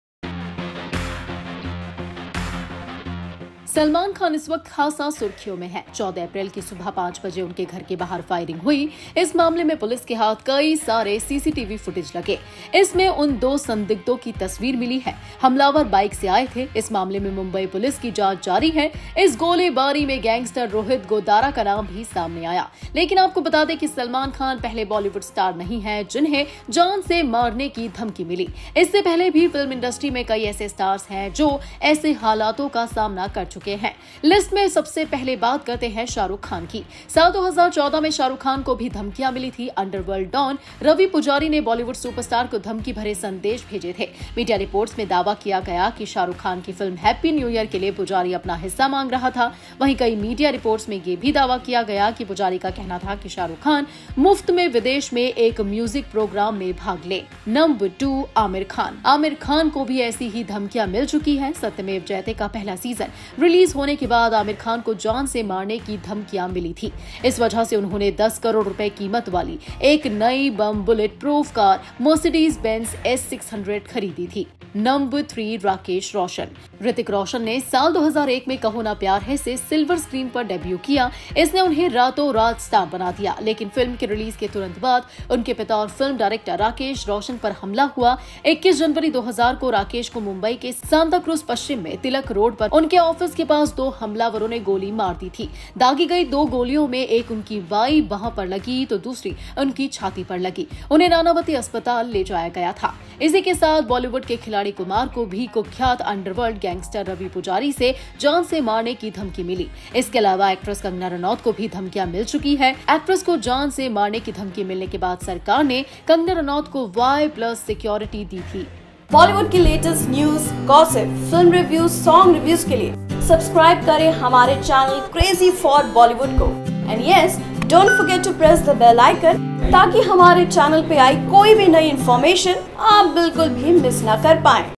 Before Salman Khan: List of other celebs who also received threats.
These 8 film stars received death threats, 1 was shot at time. check out the list.